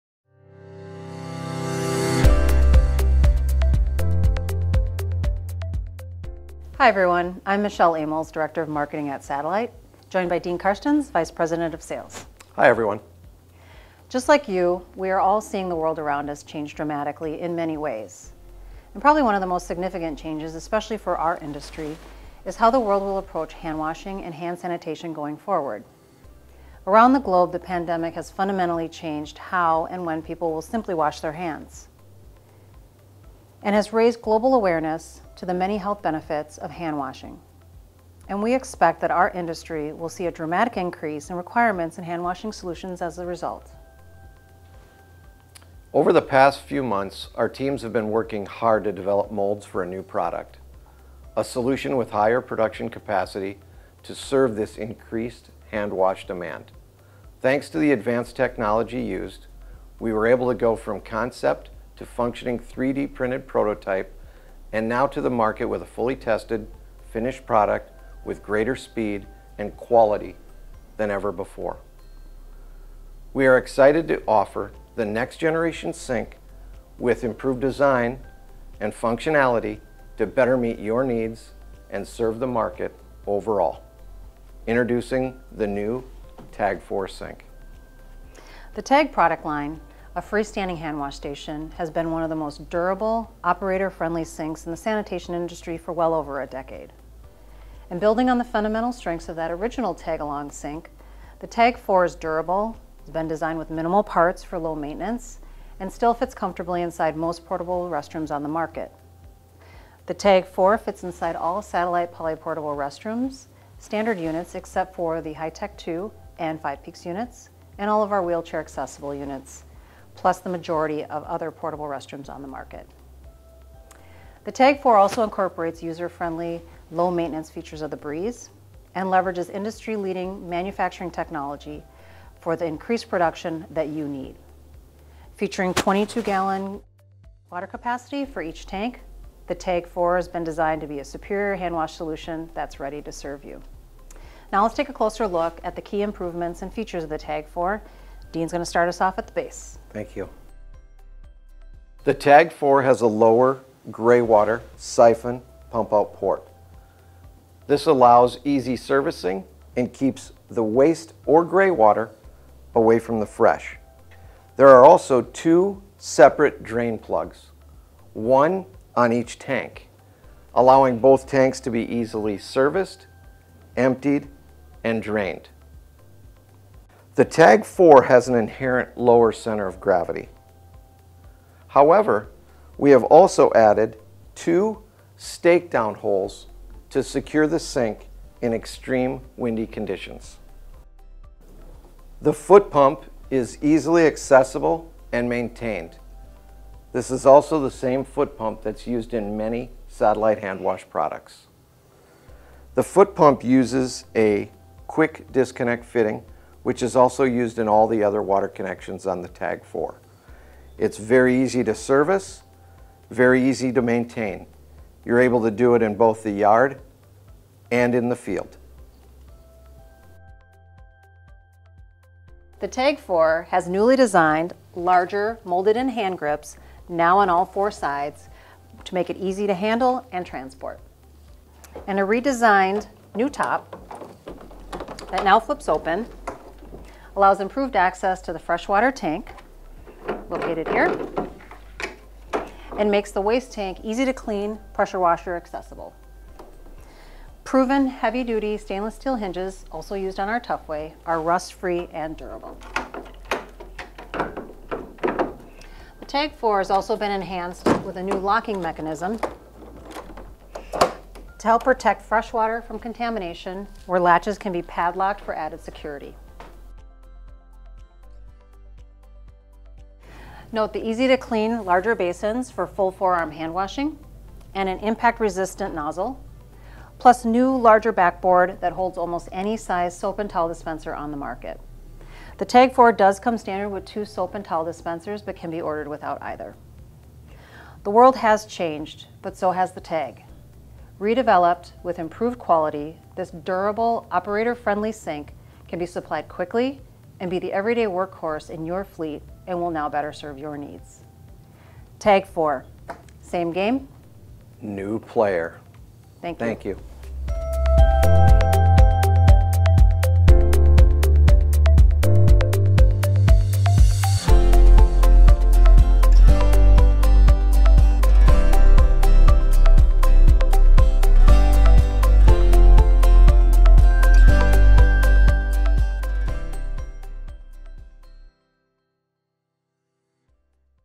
Tag 4 Product Release | Satellite Industries
The Tag 4 is the Next Generation Handwash Unit! It's durable construction and user-friendly design, make it an operators dream. Now with new technology we are able to produce the Tag 4 at rate that keeps up with the growing market demands.